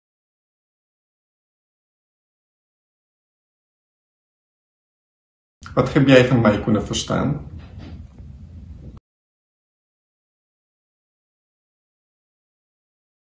Marcin Karwinkski op Werelddovendag over gelijkwaardige toegankelijkheid
Video Werelddovendag, adviseur gelijkwaardige toegankelijkheid Marcin Karwinski